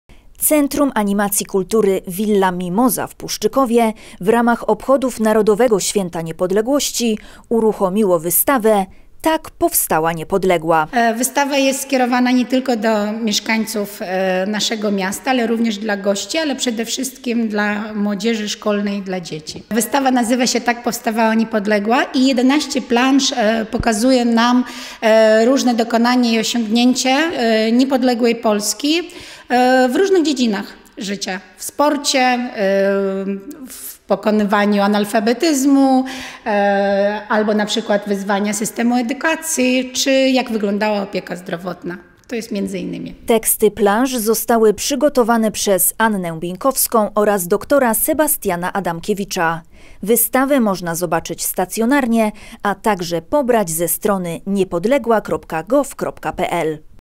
Wystawa "Tak powstała Niepodległa"
W Centrum Animacji Kultury w Puszczykowie w dniach 8-12 listopada 2021 r. można było zobaczyć wystawę “TAK POWSTAŁA NIEPODLEGŁA”.